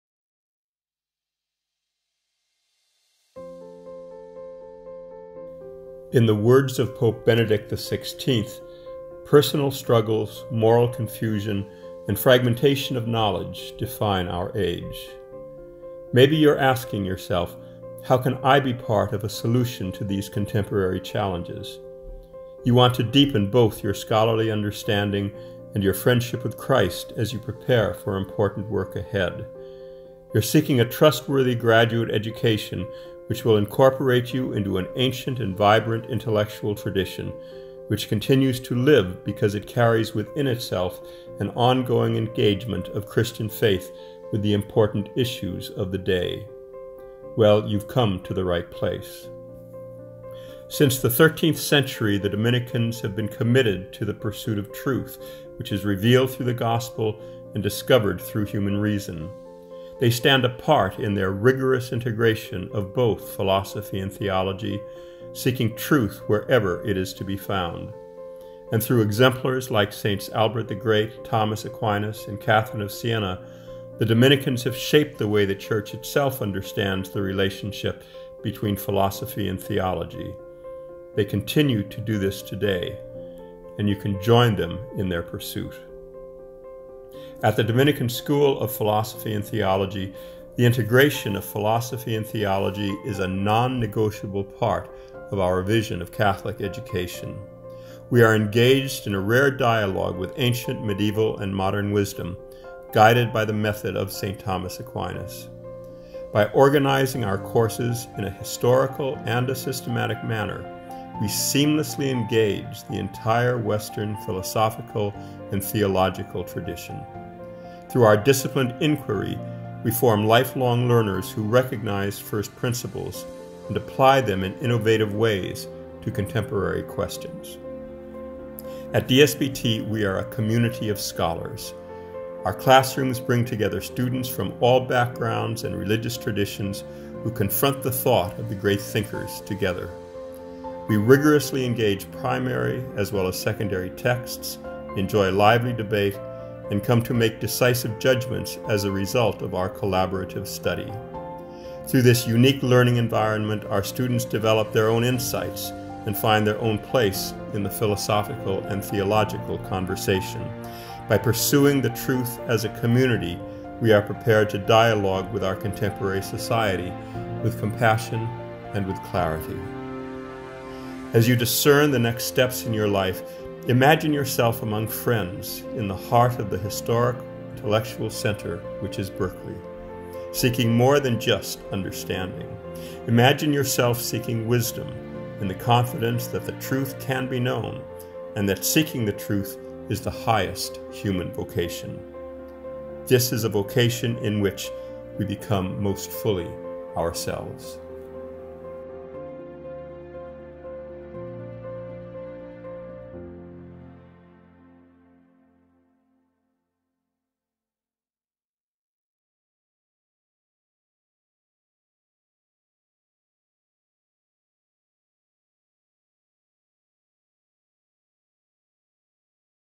Imagine Yourself Here at DSPT
Welcome to DSPT. Take a look inside at what it is like to study philosophy and theology at the Dominican School of Philosophy and Theology in Berkeley.